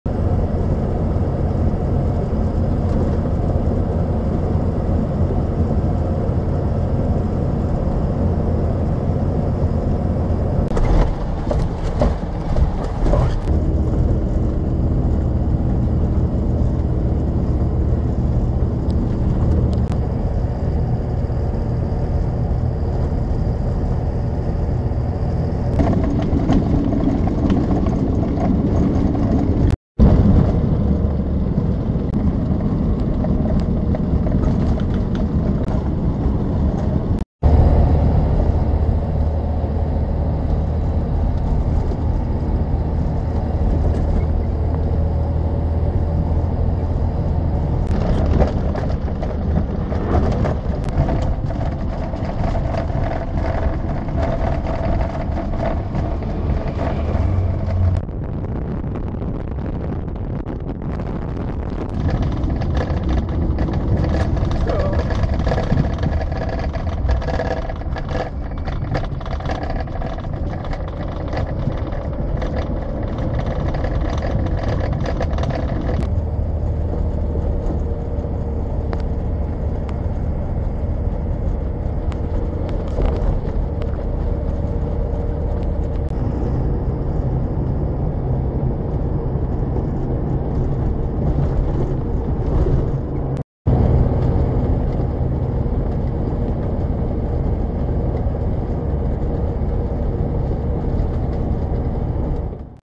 Russia - Route of bones / М56 Колыма
Route of bones, or Kolyma road, or Yakutsk-Magadan road, or M56.
Drove the road in 9 days (Yakutsk-Magadan-Yakutsk, which makes 4000 km in total). Quality of the road is good, with small exception. Filmed between 19-29 August 2014.